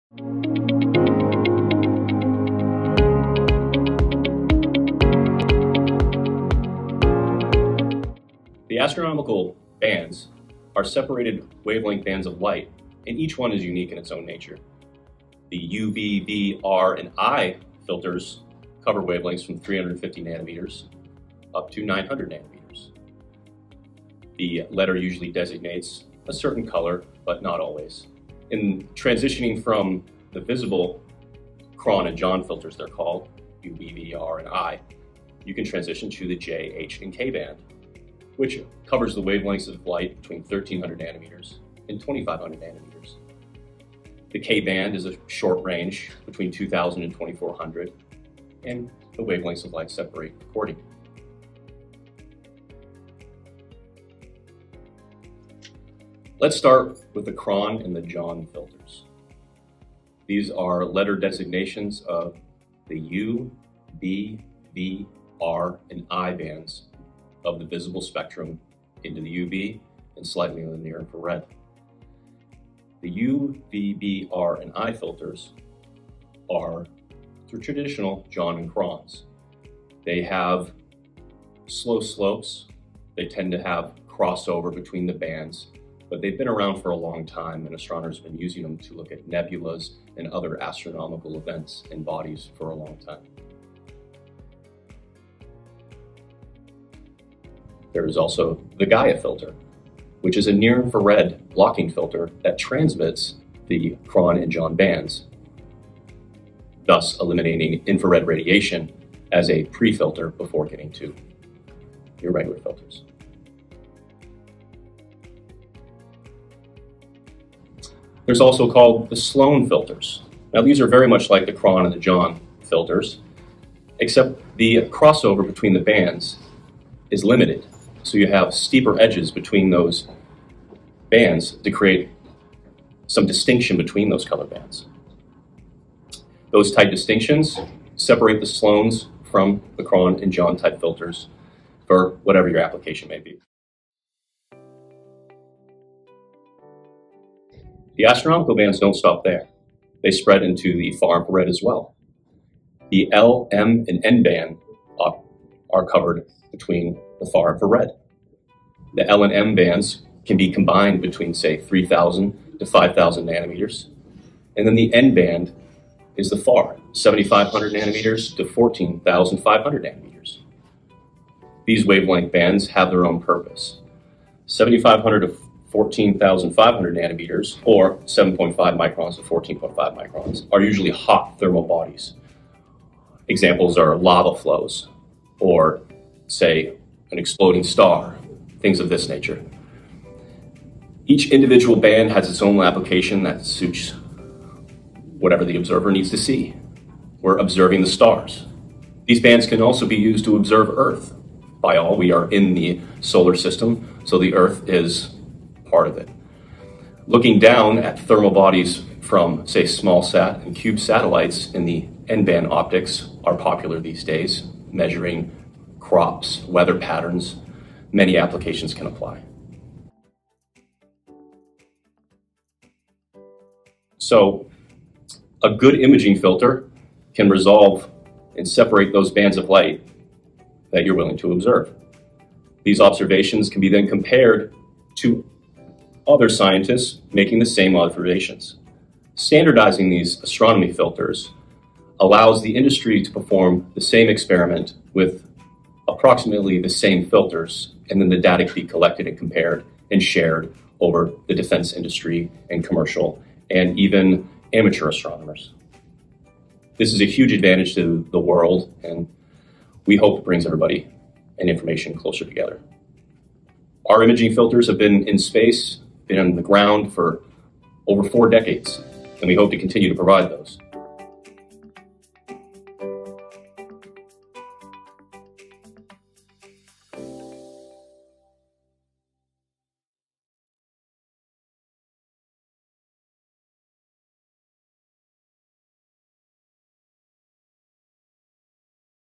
Ask Andover: How Do Imaging Filters Apply to Astronomical Observation?
In this educational video, we'll discuss the distinctions between different optical imaging filters used in astronomy. You will learn about Kron and John filters, Gaia filters and Sloan filters and how each set is used and their applications.